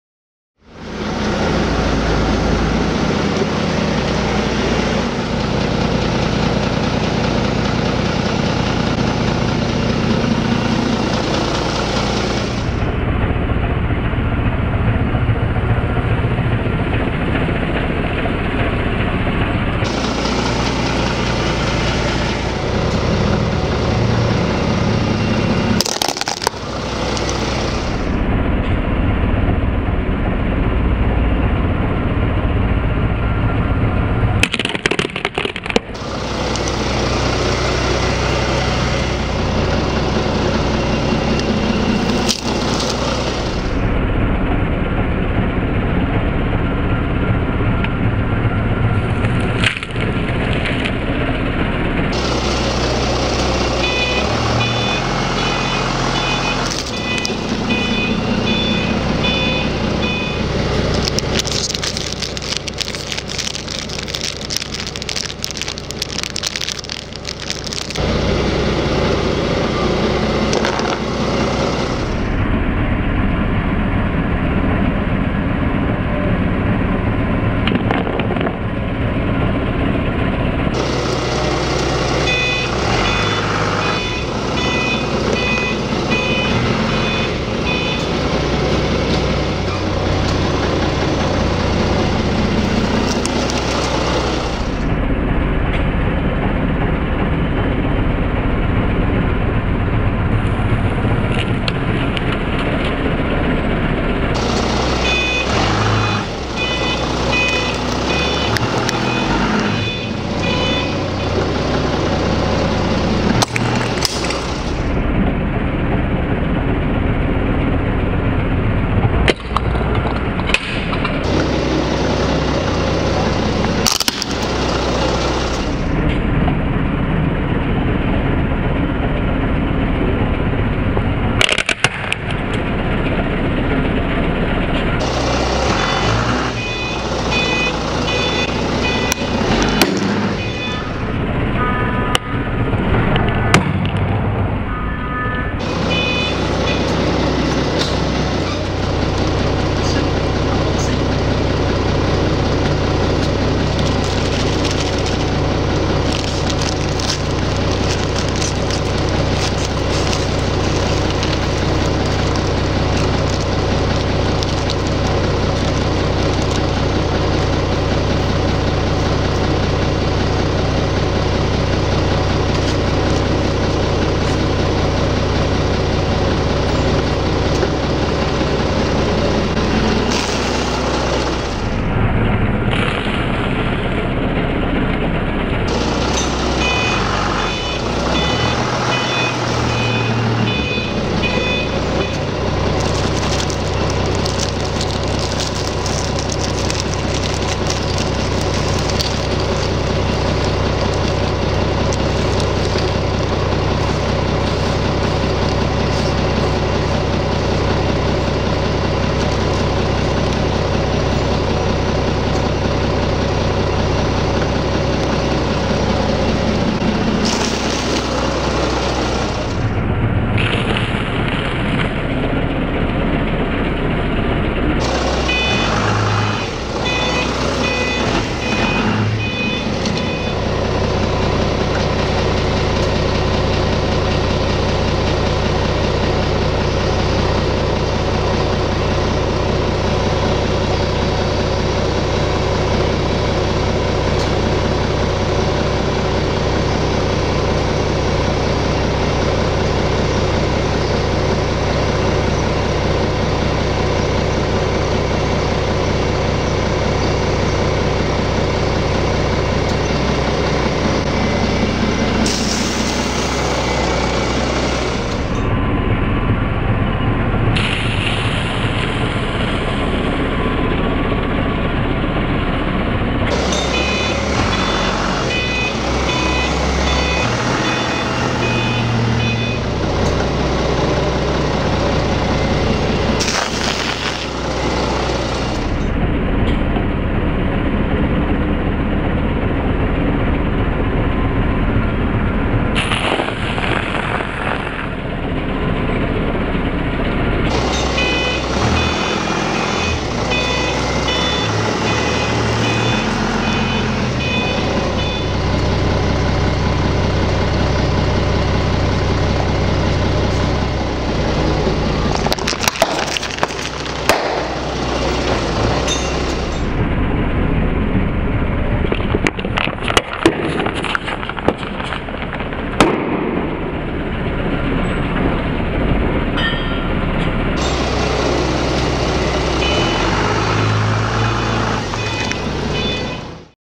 Part II EXPERIMENT:Crushing crunchy & Soft Things by Forklift
With the crush crunchy and soft things by forklift.This sound also help you achieve the relaxation.We take the food and the other things,expired and not used to make this experiments

Watch and You can say what are you feel in this comment.